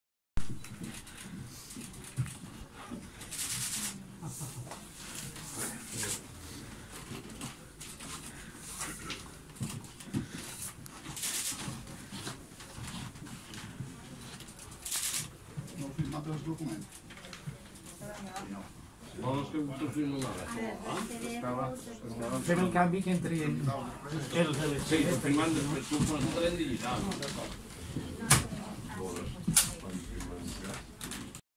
MÉS per Mallorca, MÉS per Menorca y PI presentan, con Cs y PP, el recurso contra los PGE por el
MÉS per Mallorca, MÉS per Menorca y PI presentan por segunda vez, en esta ocasión con Cs y PP, el recurso contra los PGE por el REB